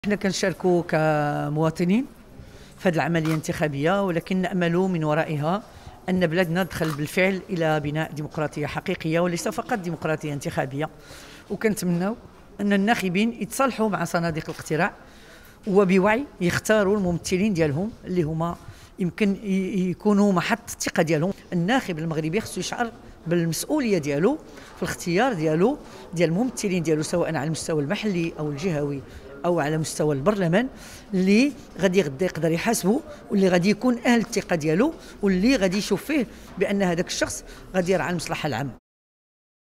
نبيلة منيب: يتعين أن تسهم استحقاقات 8 شتنبر في إرساء لبنات بناء ديمقراطي حقيقي
أعربت الأمينة العامة للحزب الاشتراكي الموحد، السيدة نبيلة منيب، عن أملها في أن تسهم الانتخابات التشريعية والجهوية والجماعية التي تجري بمختلف جهات المملكة، في إرساء لبنات بناء ديمقراطي حقيقي قادر على تخليق العمل السياسي.